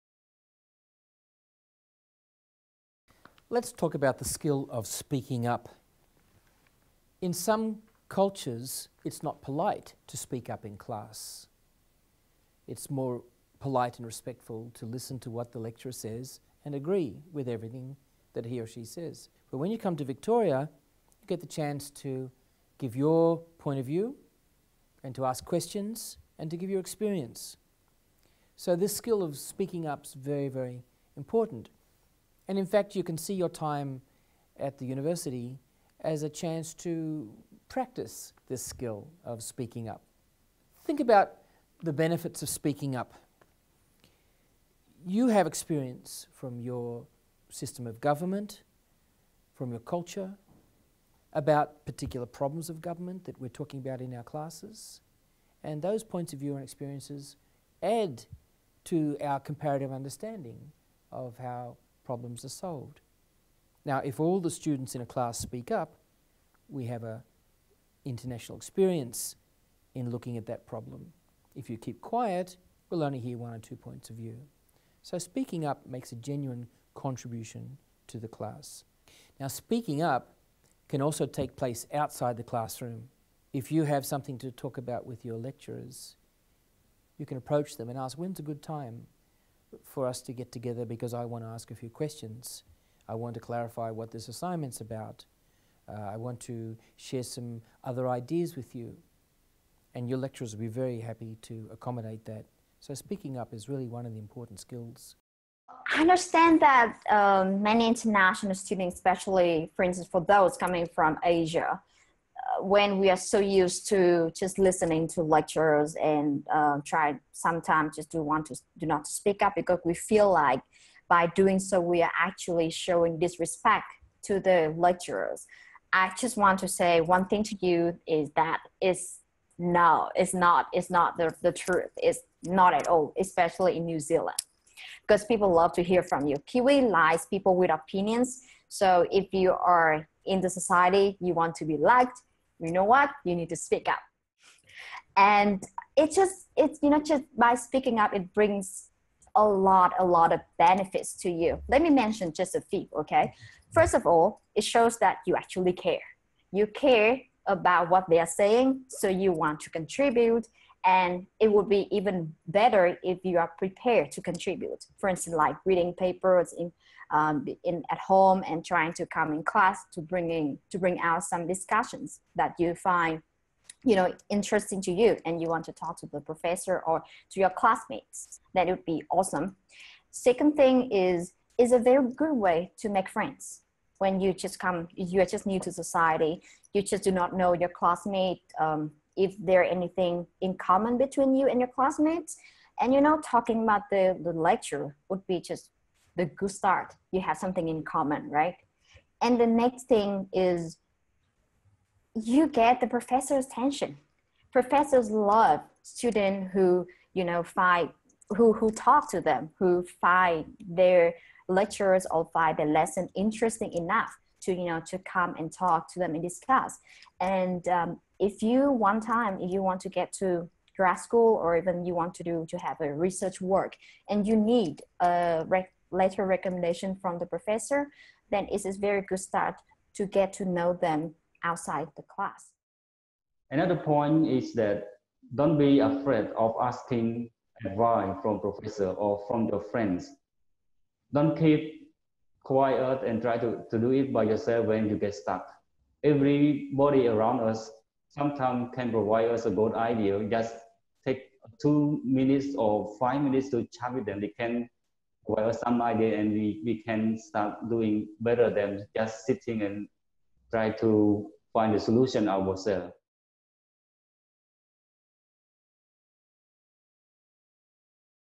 Advice for International Students Part 3 - Speaking Up
Presented by Associate Professor Graham Hassall of Victoria University of Wellington, New Zealand
Credit - The School of Government